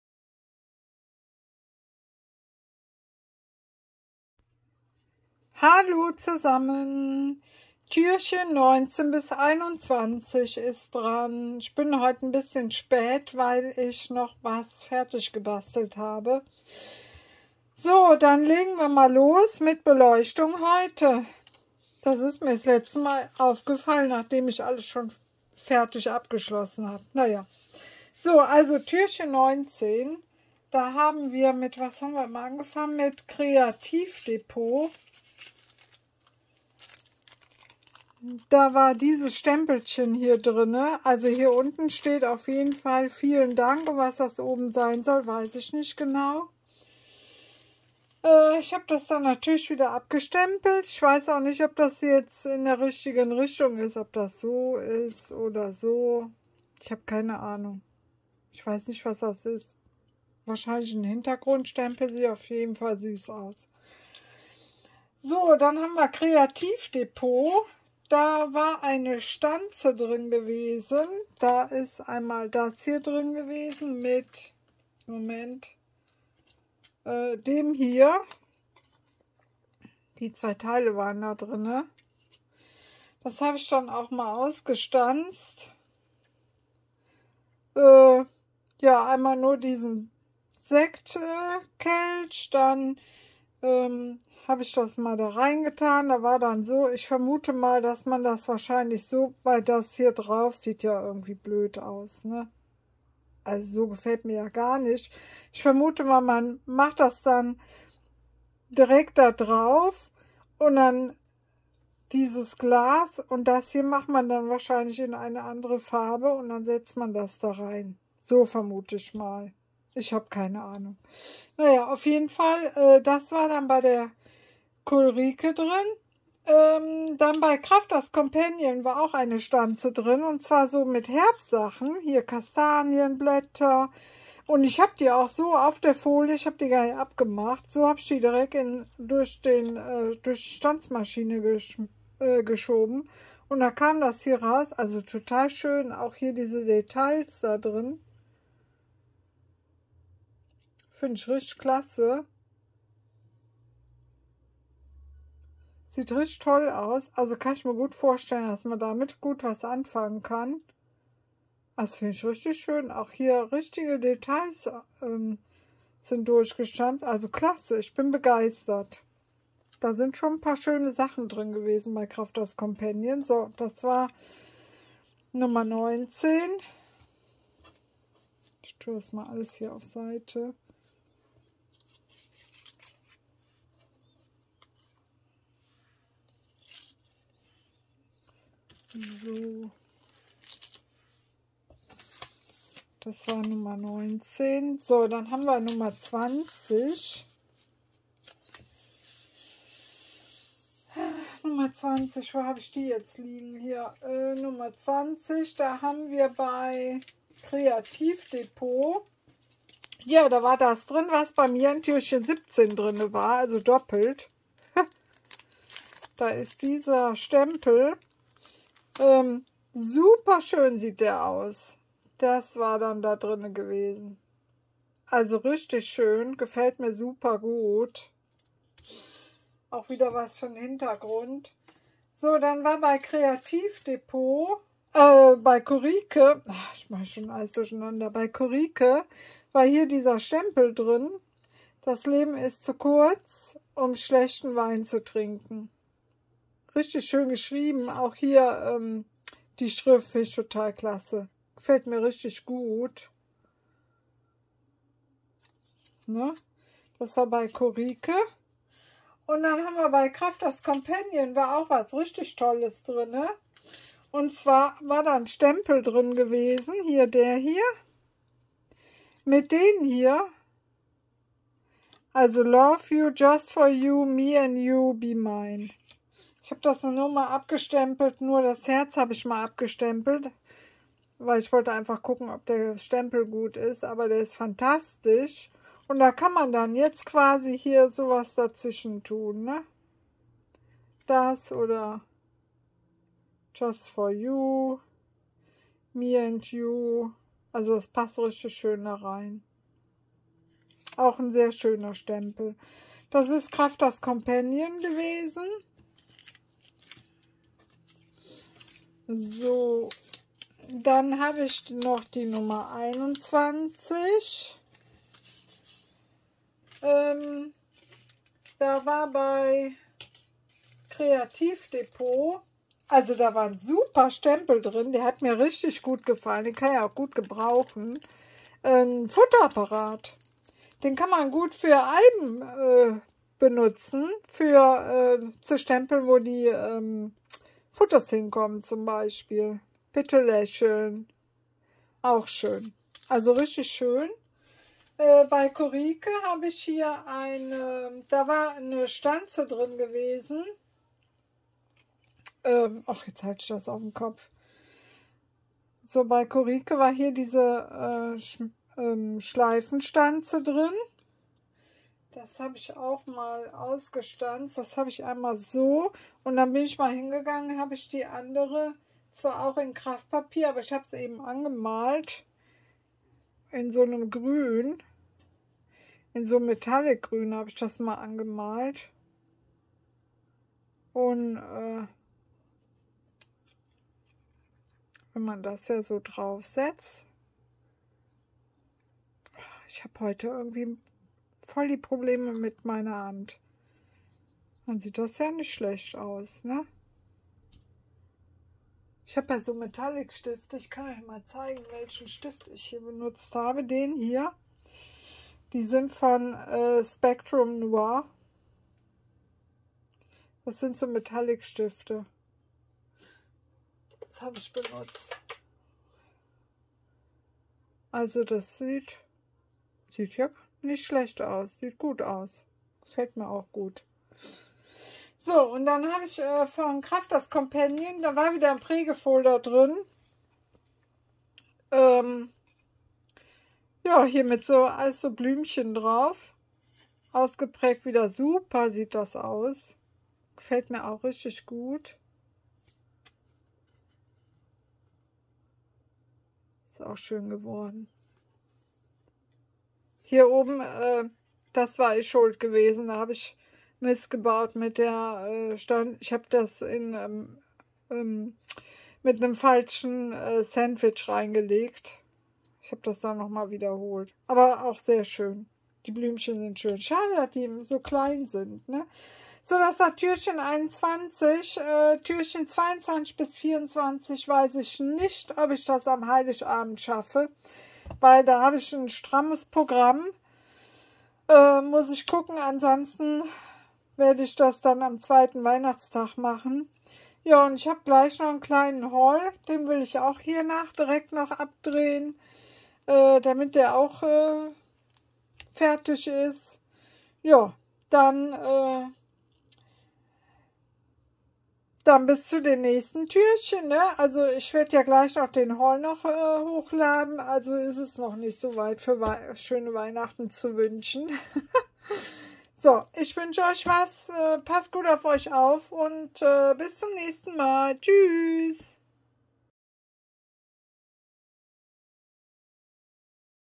Adventskalender, 19. bis 21. Türchen
Hallöle,
alle gezeigten Produkte sind von mir selbst gekauft, es handelt sich hierbei um unbezahlte Werbung.
Ich wünsche Euch ein schönes Weihnachtsfest.
Die letzten Türchen werde ich aller voraussicht am 2. Weihnachtstag zeigen.
Liebe Grüße Brigitte